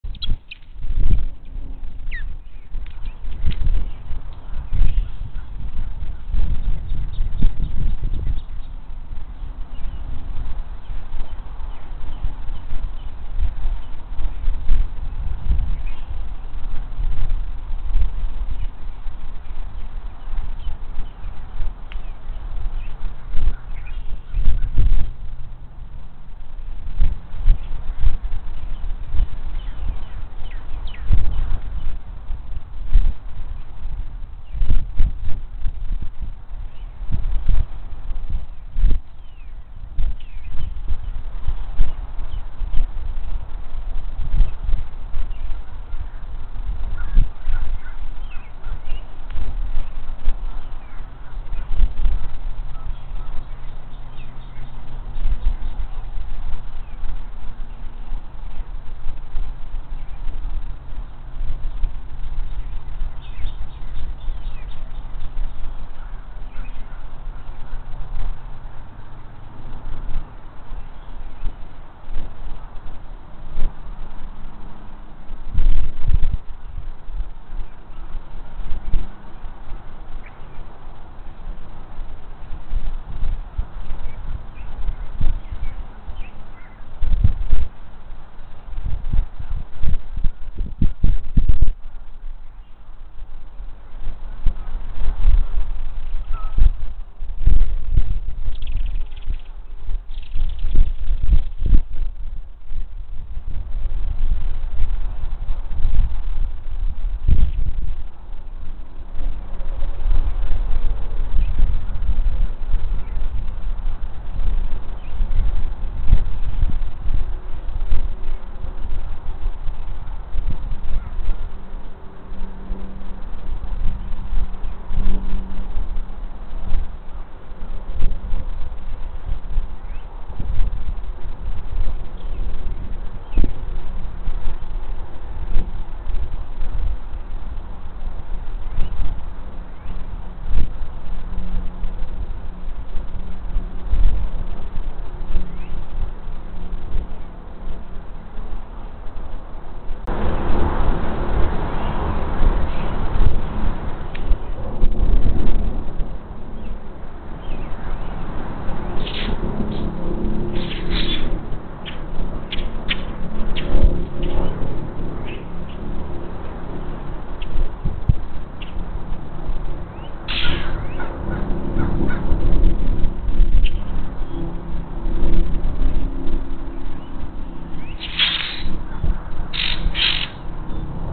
Male and Female osprey return to the Moraine Preservation Fund Osprey nest 3/17/2024
Male and Female osprey return to the Moraine Preservation Fund Osprey nest at Moraine State Park in Butler County, PA 3/17/2024